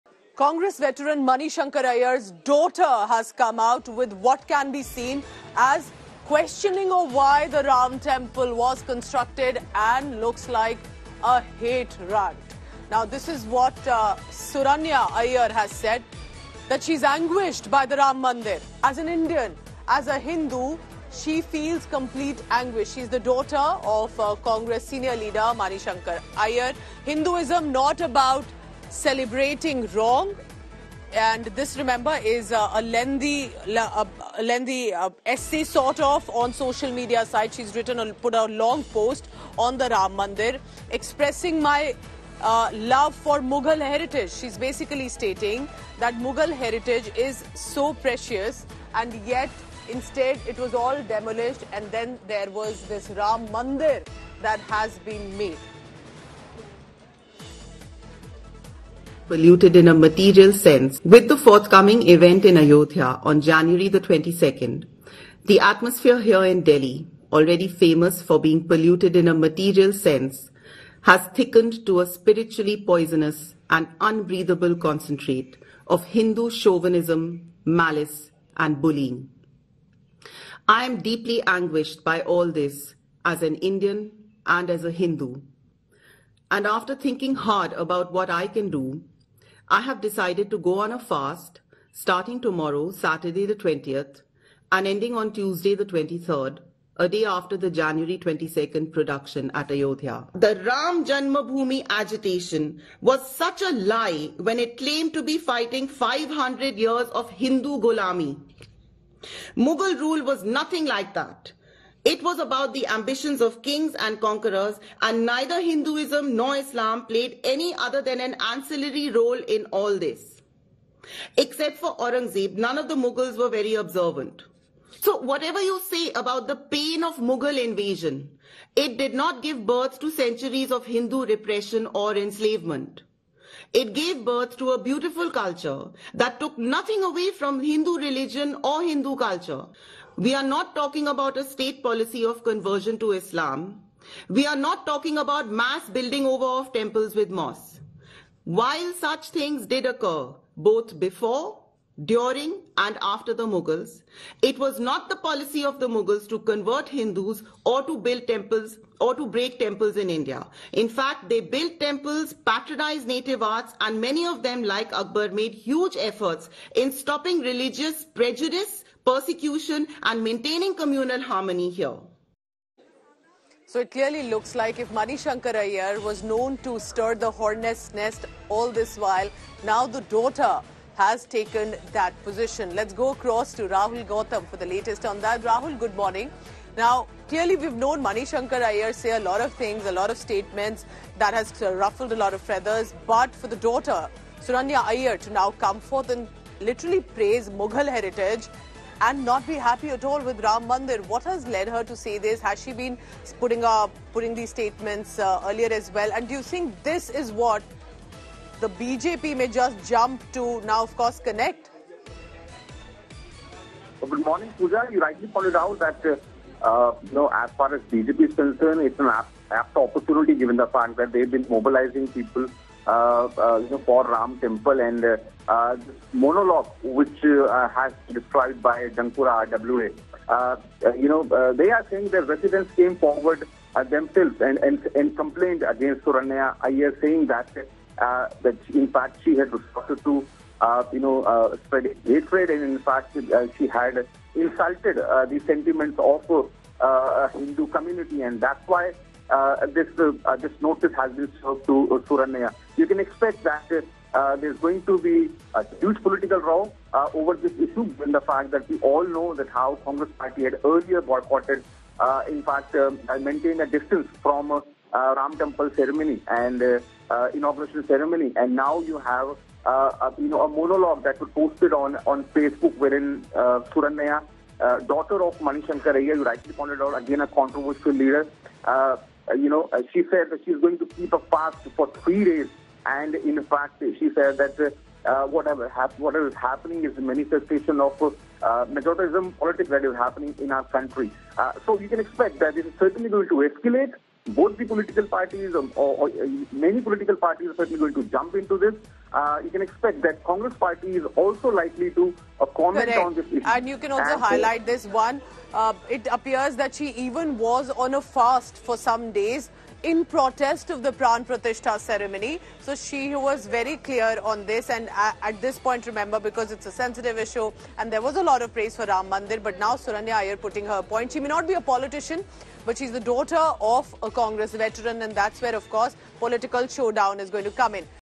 Mani Shankar Iyer's Daughter Questions Construction of Ram Mandir | Ram Mandir News
Suranya Iyer, daughter of Congress veteran leader Mani Shankar Iyer, has taken to social media to voice her disquiet over the construction of the Ram Mandir. Expressing what has been interpreted as 'anguish' and a criticism of the monument's construction, she opened up an engaging dialogue around the topic. Her views, expressed in a lengthy social media essay, profess love for Mughal heritage and question why it was demolished instead of being preserved. As an Indian and as a Hindu, she feels a deep-seated distress about the issue. Suranya's standpoint is predicted to spur conversations about the historical importance of Mughal heritage and the controversies surrounding the recent construction of the Ram Mandir. This coverage aims to offer a neutral depiction of events.